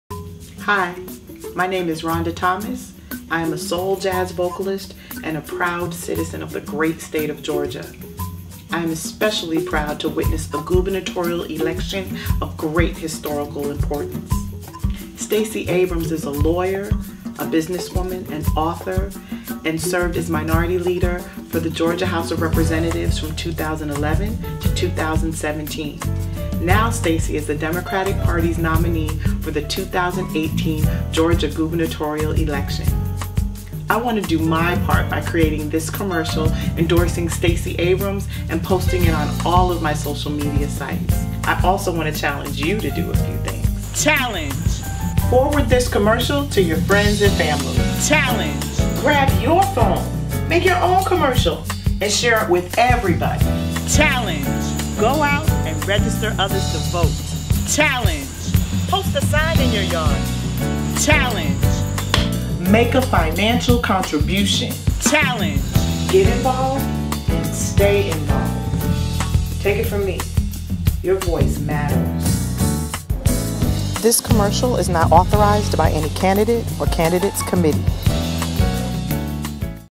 Vocalist Rhonda Thomas endorses Stacey Abrams for GA Governor 2018
Indie artist Rhonda Thomas creates a commercial endorsing Stacey Abrams for Georgia Gubernatorial race for 2018. Rhonda urges others to get involved.